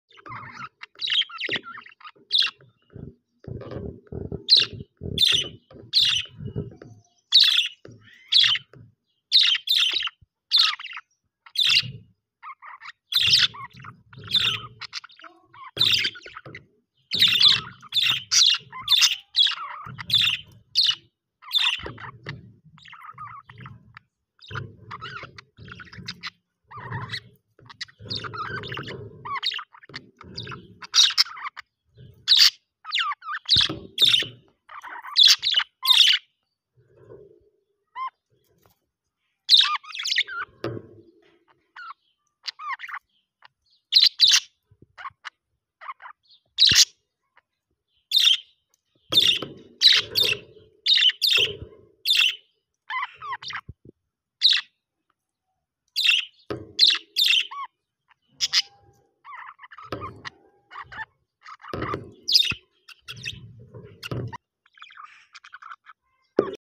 Sound of Parrots: Beautiful Parrot Voices & Budgie Sounds | Stunning Bird Calls
Stunning Parrot Voices: Beautiful Parrot & Budgie Sounds | Amazing Bird Calls

Experience the mesmerizing **sound of parrots** in this captivating video! Listen to the **beautiful parrot voices** and **budgie sounds** that will transport you to a tropical paradise. Whether you're a bird enthusiast or simply looking for relaxing bird calls, this video is perfect for you.

📢 **Features:**
- **Parrot voices** from various species
- Relaxing **budgie sounds**
- **Stunning bird calls** to soothe your mind

🌟 **Why Watch?**
- Perfect for nature lovers
- Great background sounds for relaxation and meditation

Let us know your favorite parrot sound in the comments below!